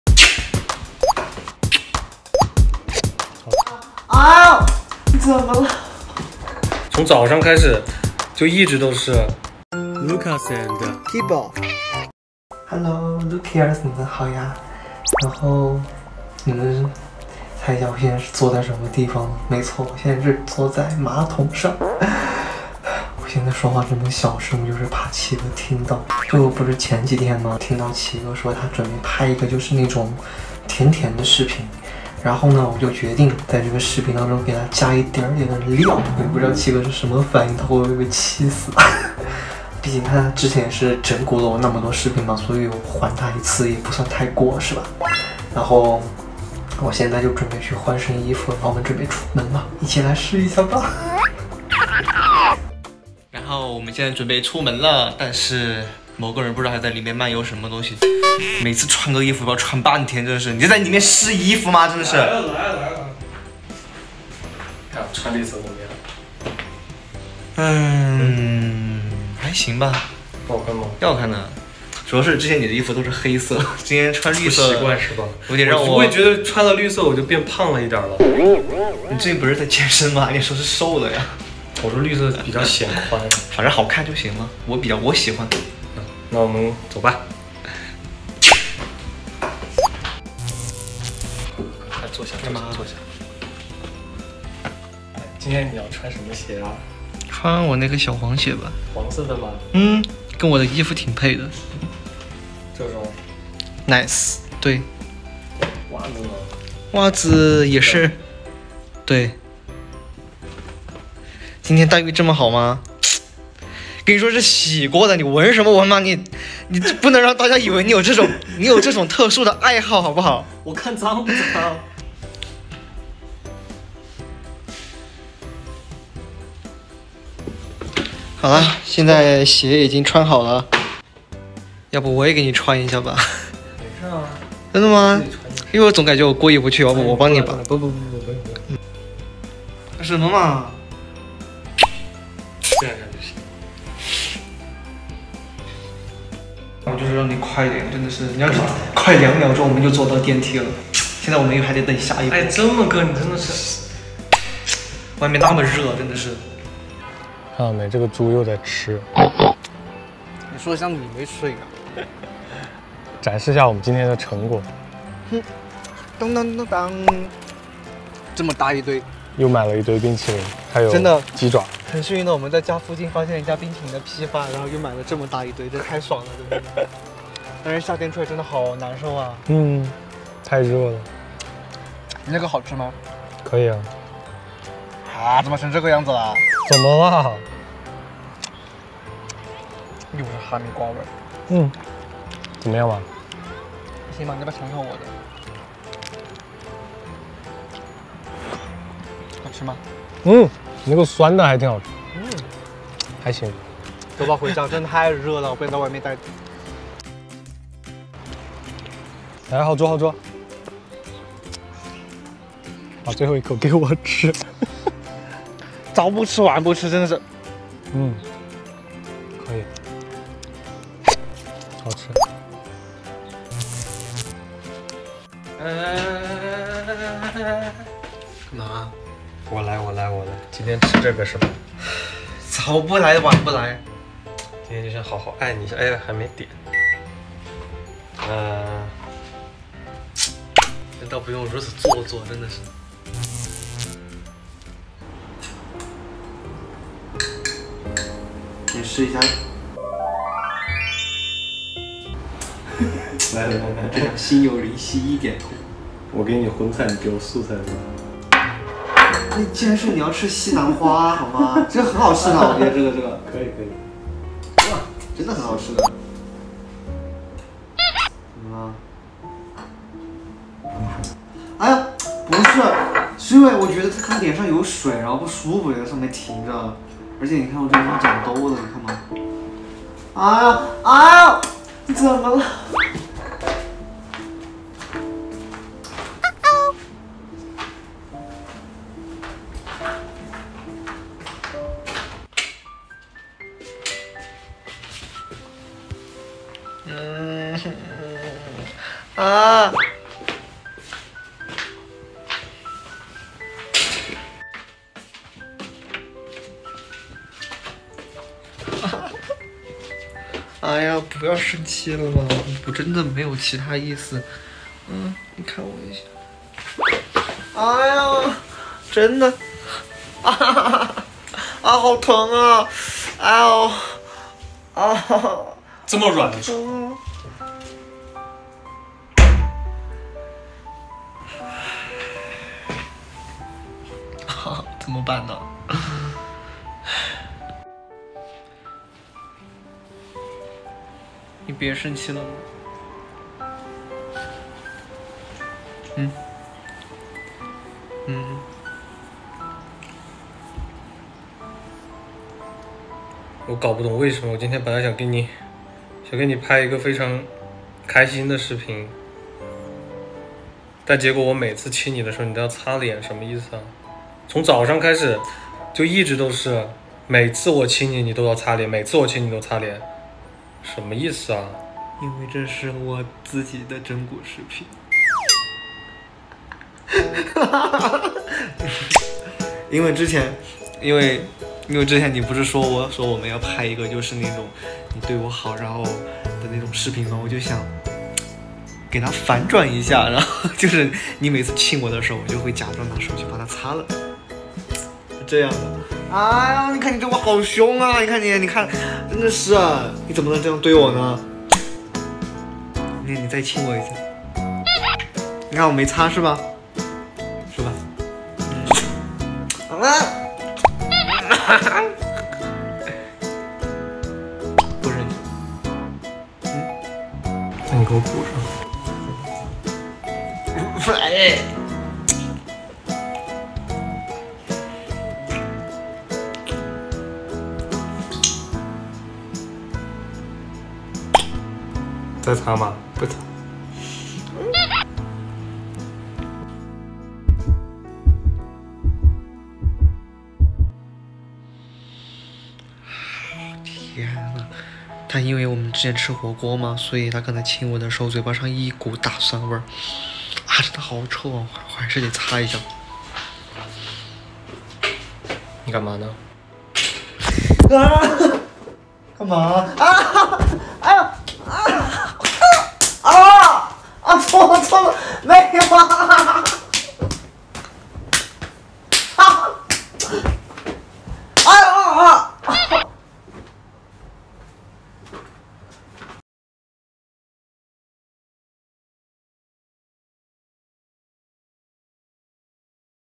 Wiping Off My Boyfriend's Kisses 💋💋💔💔**He's Angry And Ignoring Me**[Gay Couple Lucas&Kibo BL]
Kibo's Revenge video! Do you think Kibo has succeeded this time?😂
Members of this channel can enjoy the following benefits:
成為此頻道的會員即可獲享以下福利：